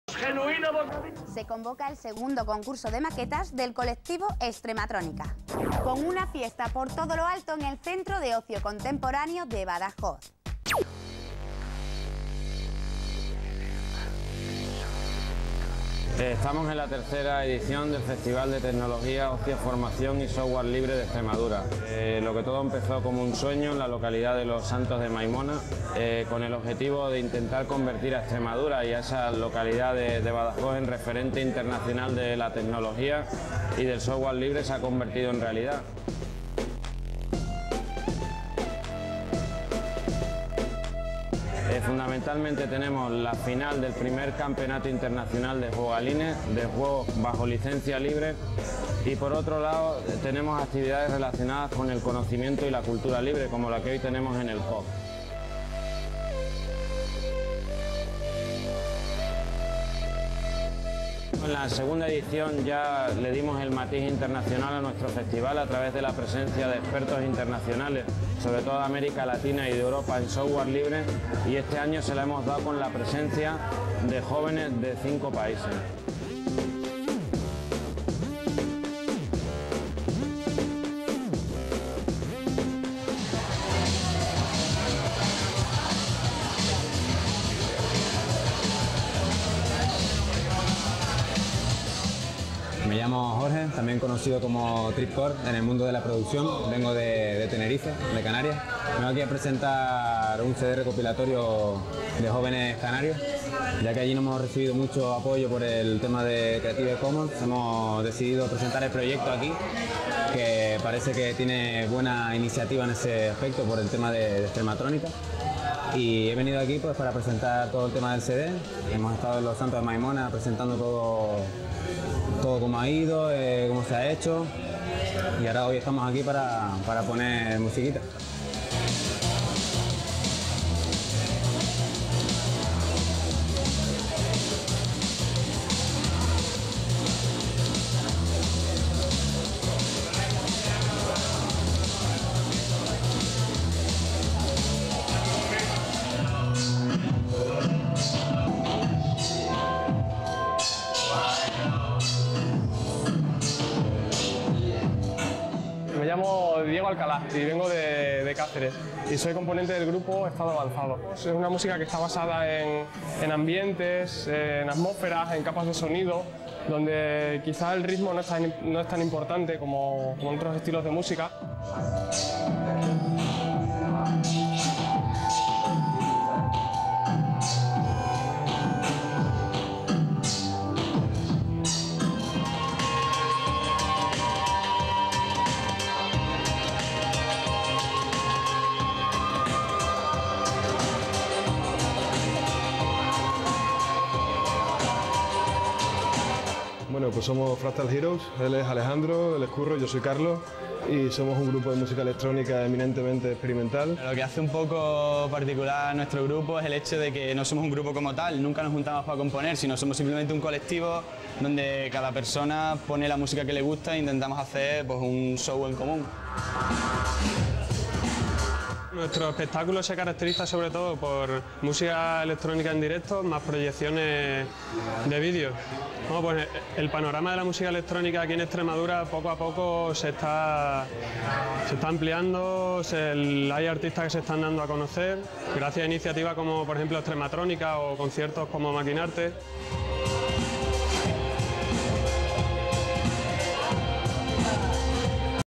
Extrematronica en el COC
Fiesta Extrematronica organizada por la  asociacion juvenil ALTERNATIVA JOVEN en el Centro de Ocio Contemporaneo de Badajoz. Fué un verdadero éxito las actuaciones de Fractal Heroes, Estado Avanzado, Trippcore y Supersexy Sound System & Maese Draven